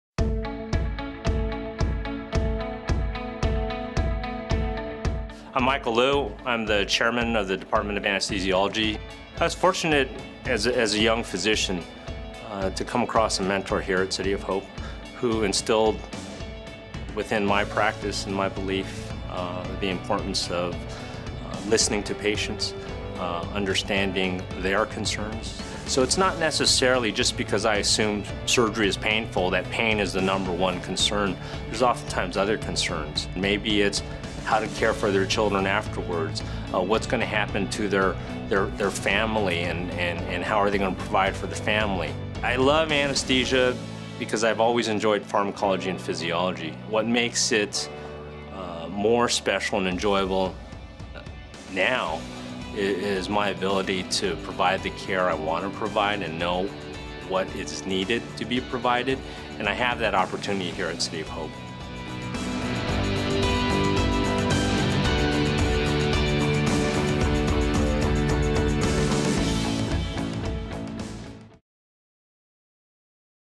Meet Michael W. Lew, M.D. | City of Hope
Dr. Lew is the Chair of Anesthesiology and clinical professor at City of Hope near Los Angeles, CA. Read his profile Learn more about the Department of Anesthesiology and its services